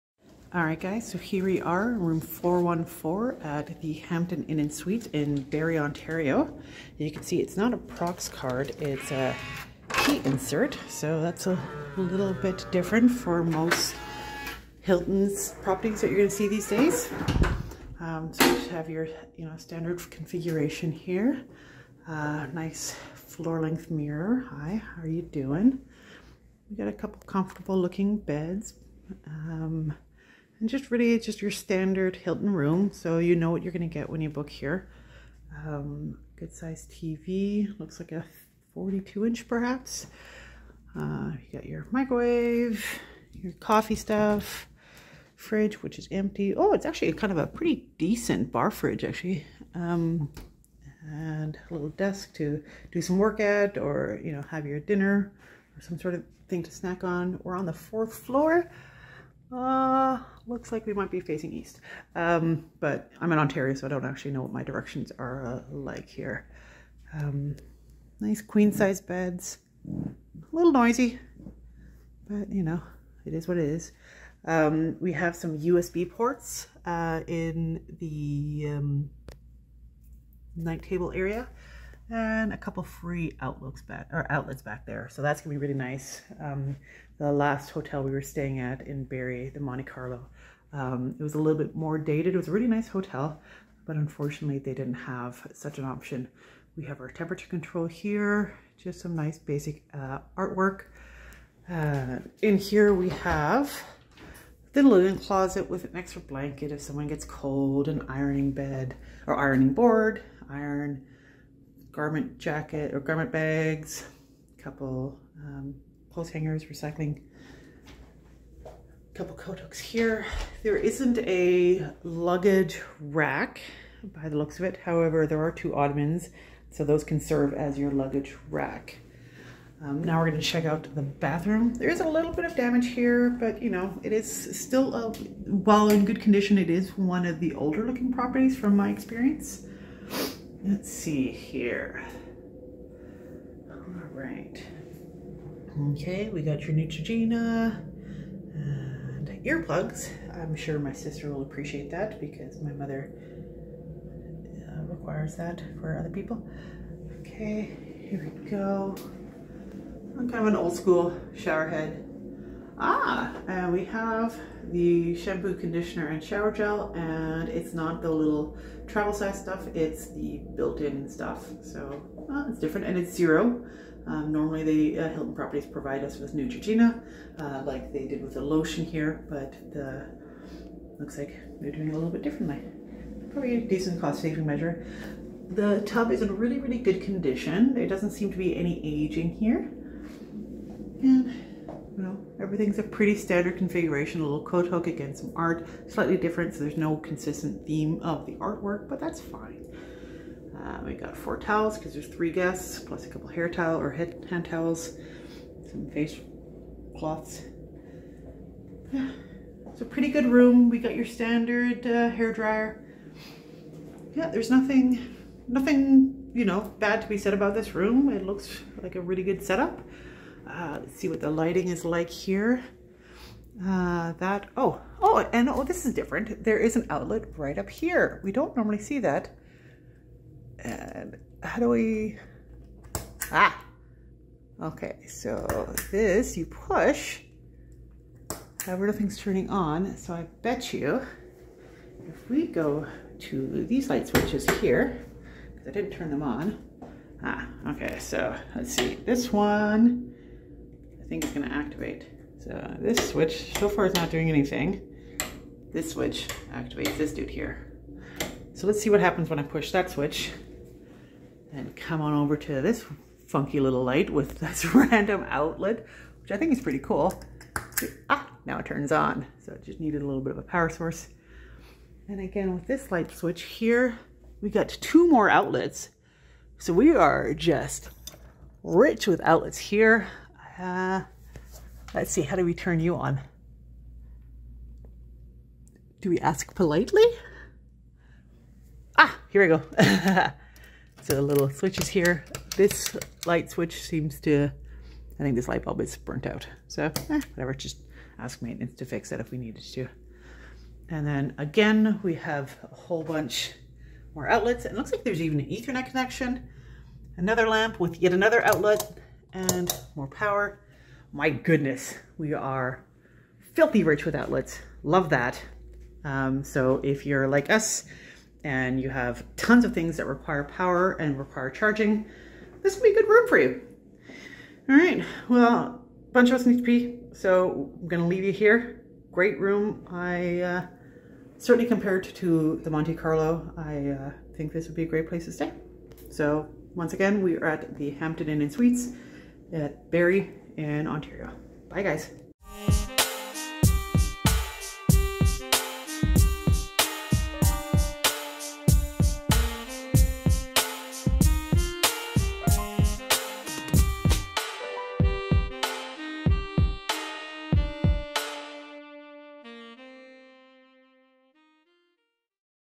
Hampton Inn & Suites Barrie Ontario
Hampton Inn & Suites Barrie Ontario. At the Hampton Inn & Suites in Barrie, Ontario we show you a guestroom complete with 2 Queen size beds, 37" HD flat screen TV, and complimentary high speed internet service.

VIDEO TIMESTAMPS - Hampton Inn & Suites Barrie Ontario
00:00 Entrance
00:25 Room
02:15 Bathroom
03:38 Lights And Power
05:12 Final Thoughts